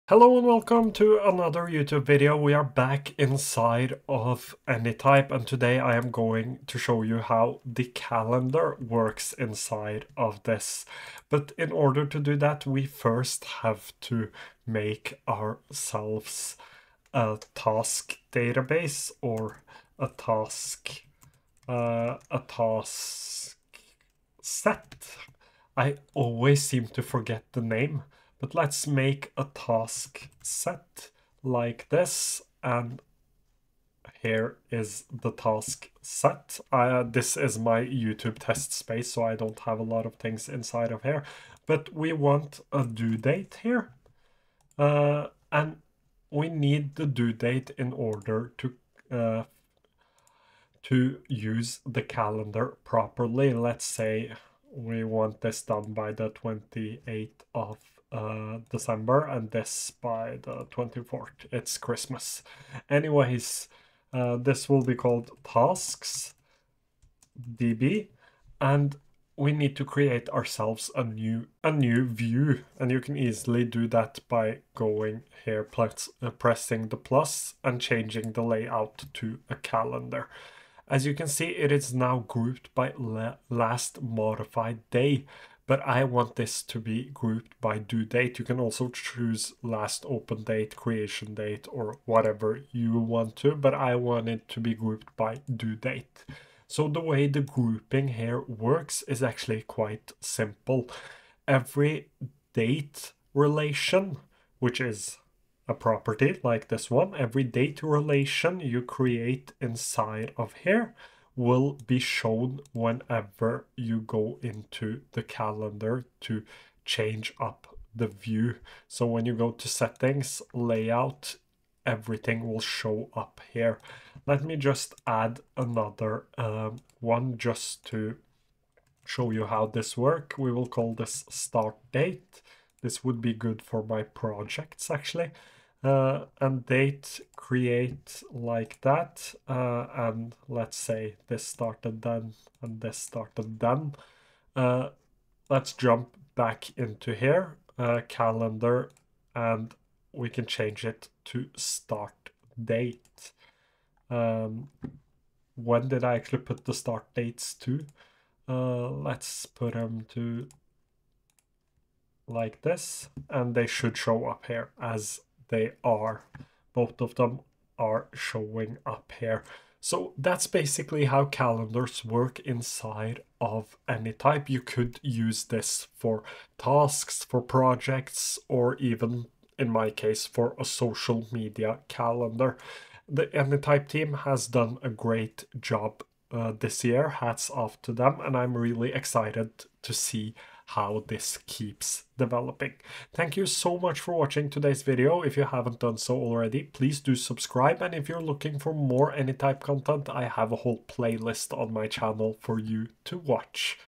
How to use Anytype calendar
In todays video we are back inside of Anytype. Today we are going over the calendar. How it works and how you can use it.